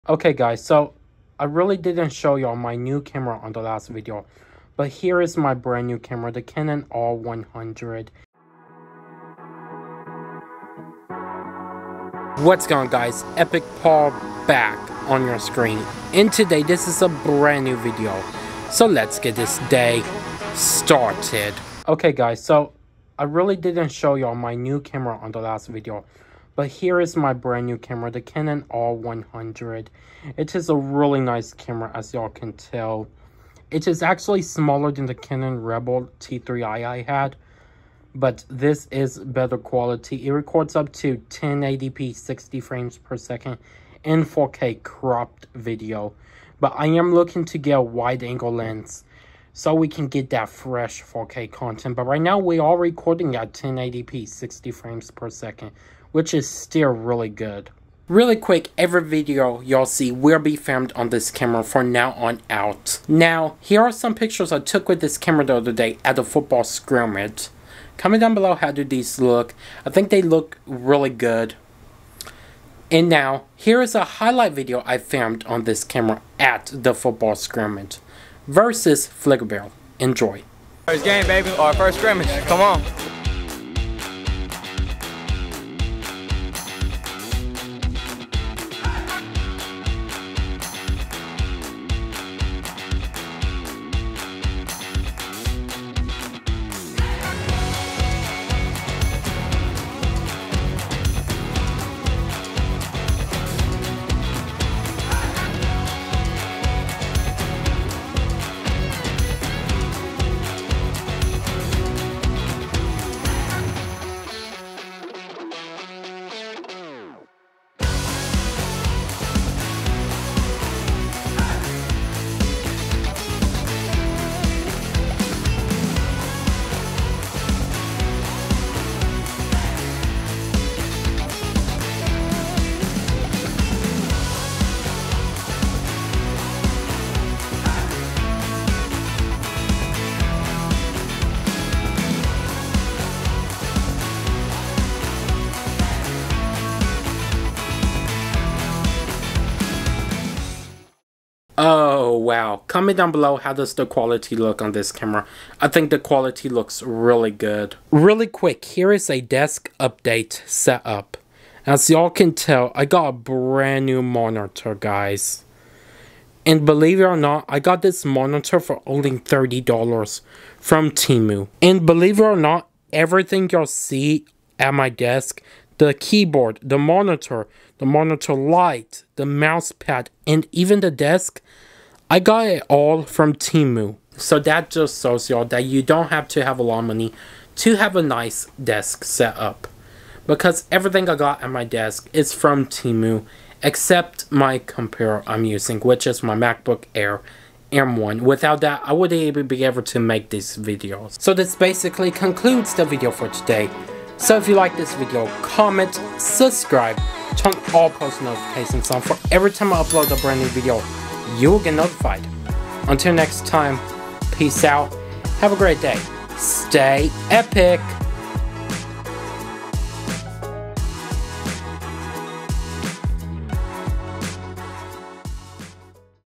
My New Camera & Football Scrimmage Highlights!
Join me as I showcase my new camera and dive into some intense football scrimmage highlights. Watch as I put the camera to the test, capturing all the action and excitement from the field.

                          Video Information

Video Information - My New Camera & Football Scrimmage Highlights

                 Recorded With - Canon R100

              Upload Date - September 1, 2024